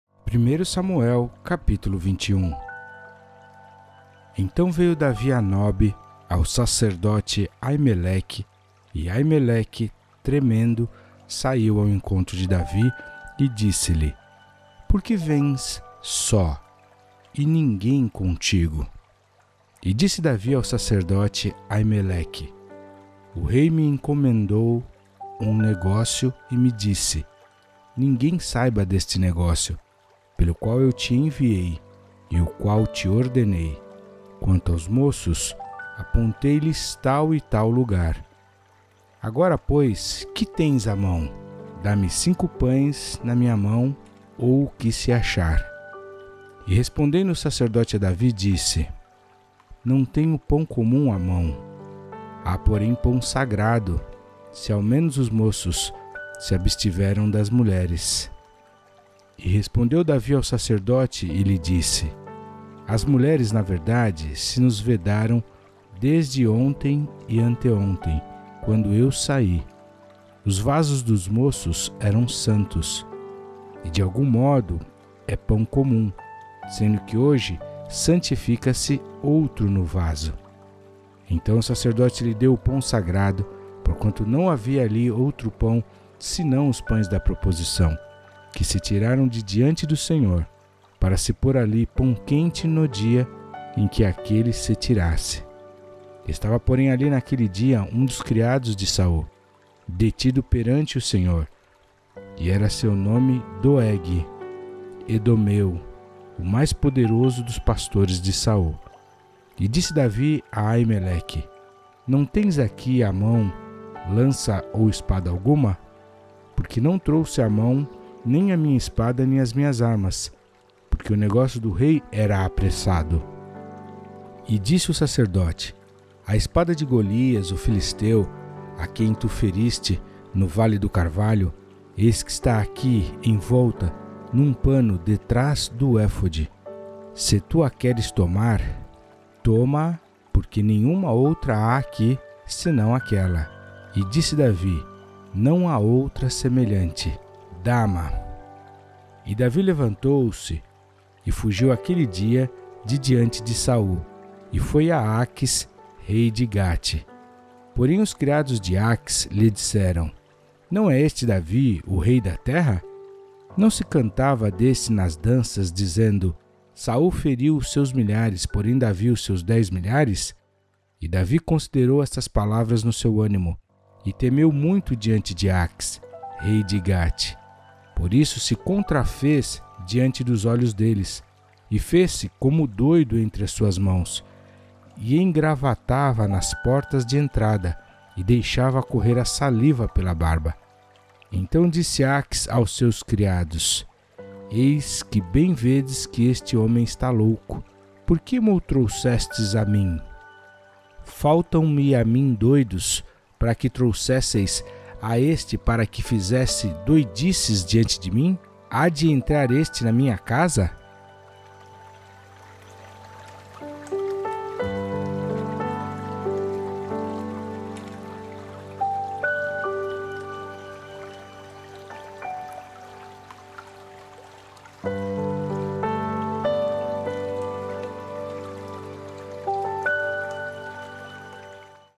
1 SAMUEL 21 📖 - Bíblia JFA Offline
Separe tempo e lugar para meditar na palavra de Deus. // 🆘 SOS VIDA: e se você precisa de ajuda ou atendimento pessoal, contate nossa equipe através do WhatsApp

0:00:04 - Davi come os pães consagrados
0:02:30 - Davi finge ataque de nervos